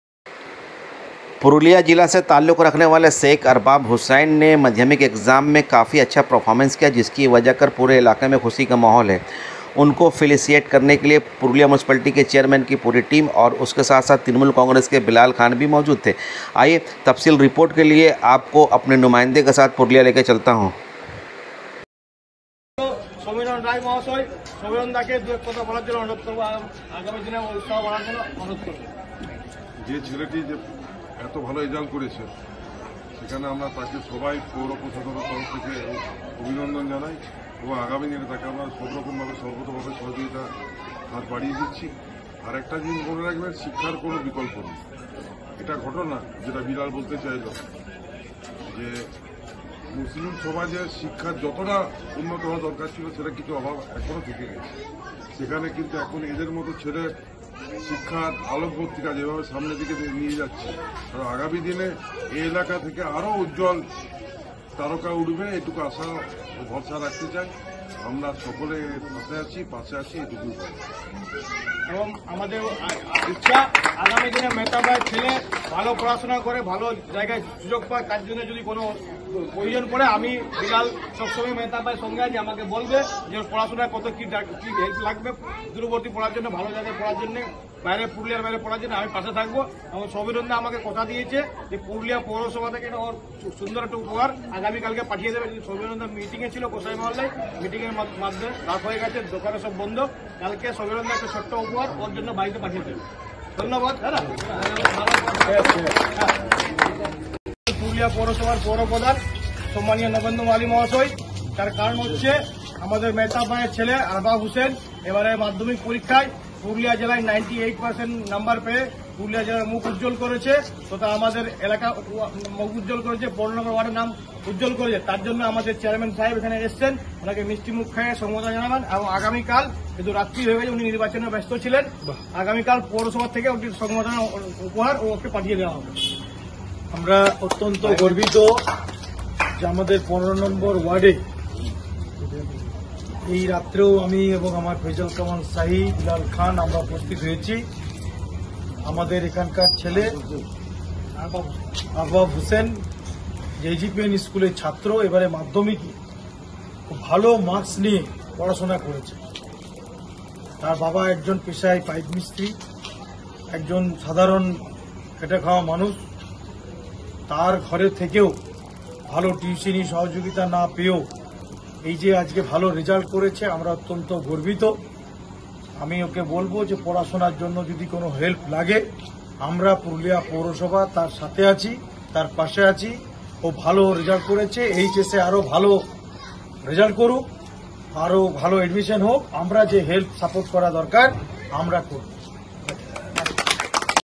Sk Arbab Hussain Get Excellent Marks In MP 2024 Examination
Present chairman Purulia municipality, Chairman in council member samiran Roy and Bilal Khan General Secretary Purulia District trinamool youth Congress Felicitation To Sk Arbab  Hissain Who Get Excellent Marks In MP 2024  Examination